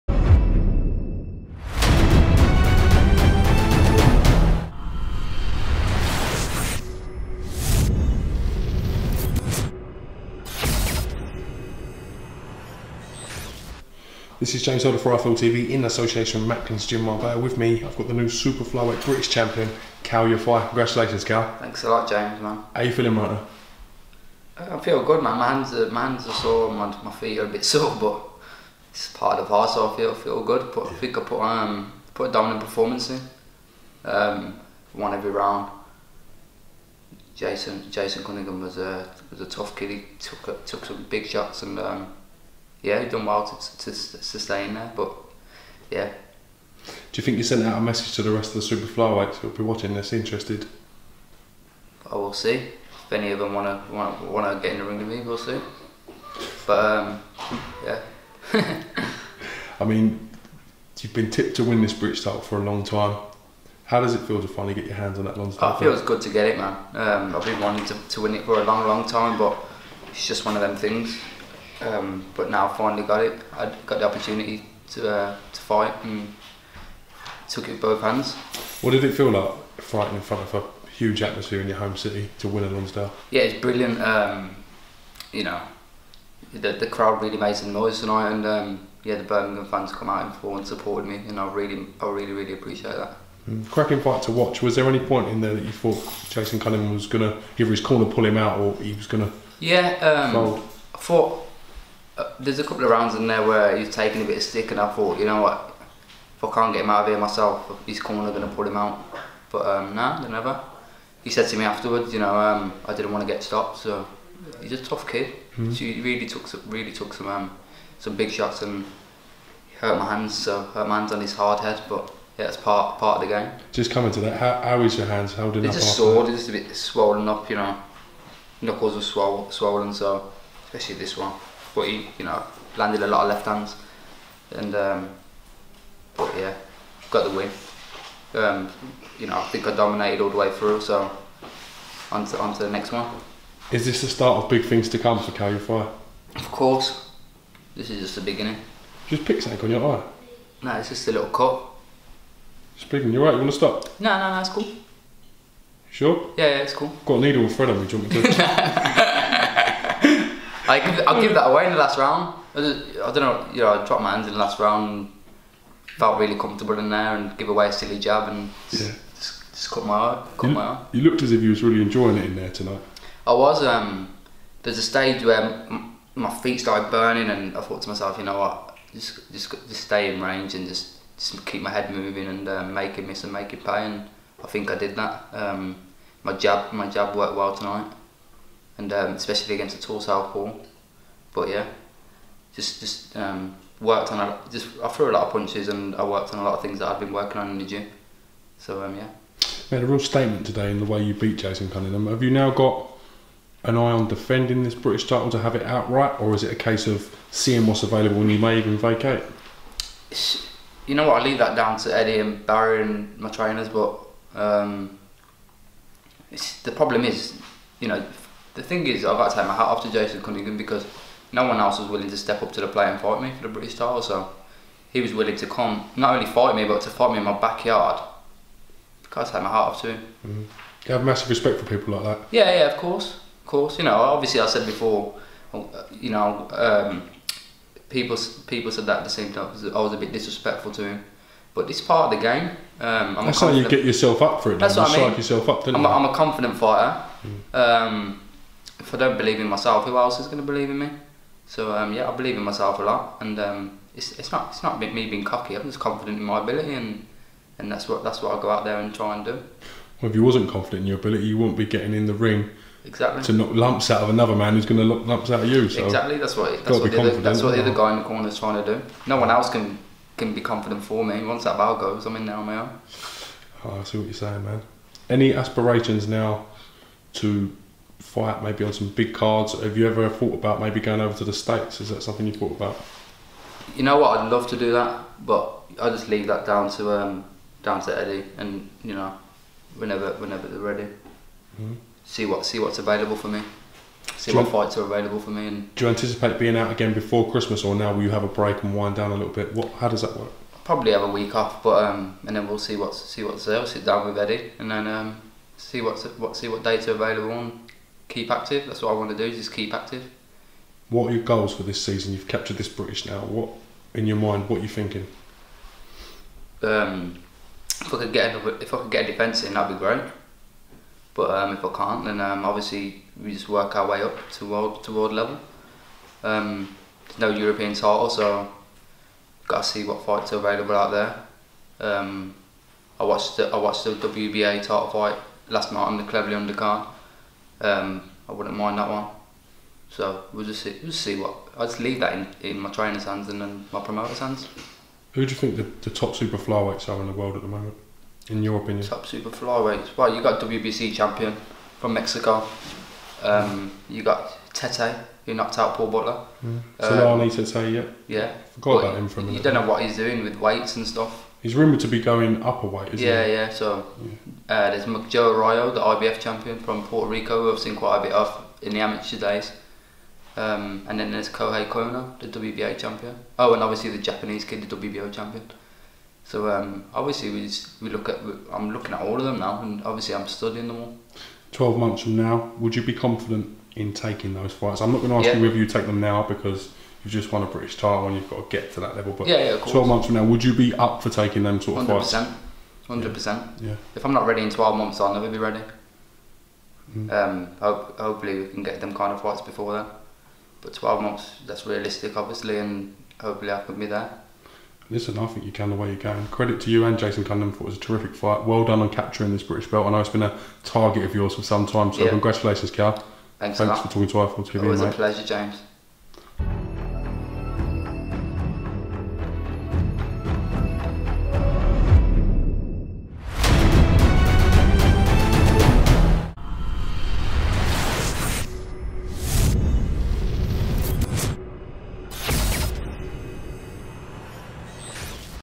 KAL YAFAI EXCLUSIVE AFTER HE CAPTURED THE BRITISH TITLE - 'THIS IS JUST THE BEGINNING
KAL YAFAI EXCLUSIVE AFTER HIS CAPTURED THE BRITISH TITLE - 'THIS IS JUST THE BEGINNING' -   (Video was corrupted after some work its been thankfully restored sorry Kal Yafai for the delay )